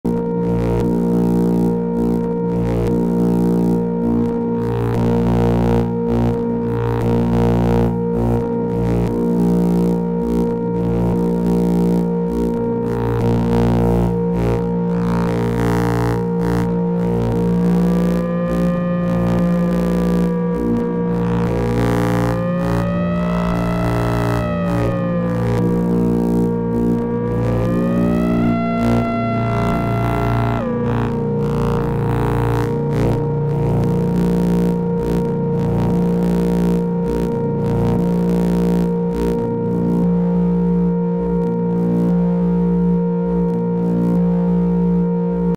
Buchla Textures in Eurorack
Follow up video from my last one.. caught what I feel is the sweet spot.. designed to be played loud in monitors on in headphones